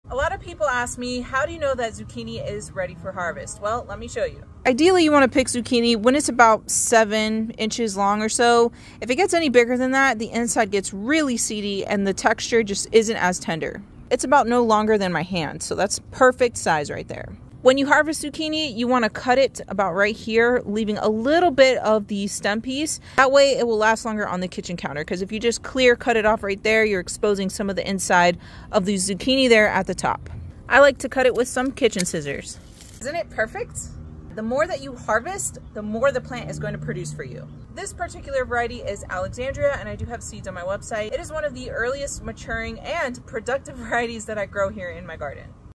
When & How To Harvest Perfect Zucchini
🌱 Harvesting Zucchini: when and how to harvest
Zucchini is at it's best when they're harvested at the right time. How do you know when they're ready?
Look for these signs:
👉 Size Matters: Zucchini is best harvested when they're 6-8 inches long. Once they get bigger, they can become tough and less flavorful.
👉 Firmness: Gently squeeze the zucchini; it should be firm and not easily dented.
👉 Shiny Skin: The skin should be glossy and vibrant, not dull or wrinkled.

Harvesting Techniques:
1️⃣ Use a sharp knife or scissors o cut the zucchini from the stem, leaving a small piece of stem attached. This prevents the zucchini from rotting at the end.
2️⃣ Avoid pulling or twisting the zucchini off the plant, as it can damage the plant and affect future harvests.
3️⃣ Harvest regularly: The more you pick, the more the plant will produce! Keep a close eye on your zucchini plants, and don't let them become oversized.

Find the same gardening supplies I use in my own garden by following my Amazon